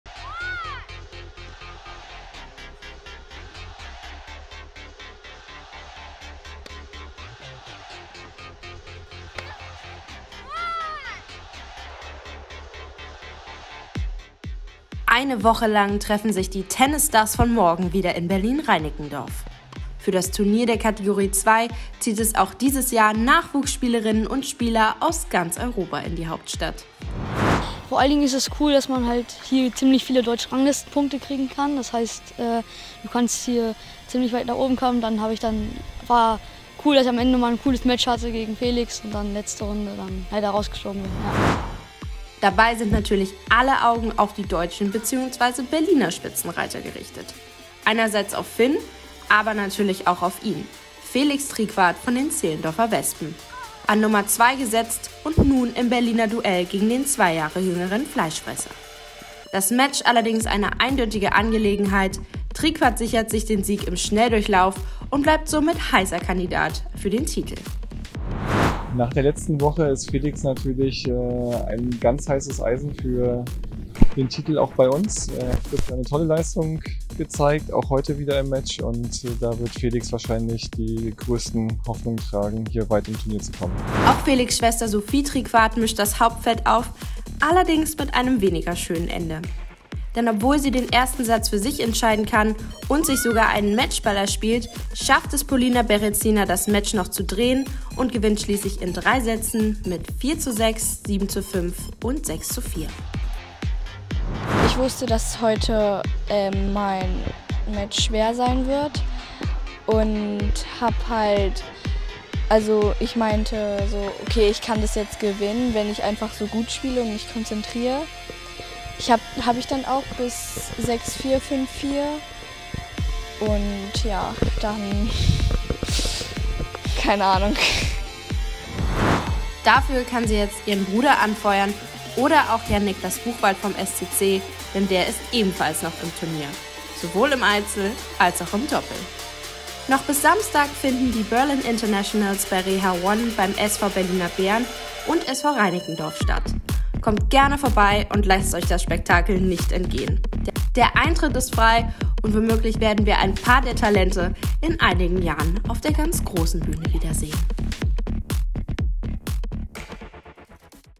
Berliner TENNISSTARS mischen mit! I Berlin Internationals by REHA ONE I TE U14 Cat. 2
Über 100 Spielerinnen und -spieler - 20 Nationen - sechs Tage Tennis und ein Titel - das sind die Berlin Internationals by REHA ONE!

Eine Woche lang treffen sich die Tennisstars von morgen wieder in Berlin Reinickendorf. Für das Turnier der Kategorie 2 zieht es auch dieses Jahr Nachwuchsspielerinnen und -spieler aus ganz Europa in die Hauptstadt.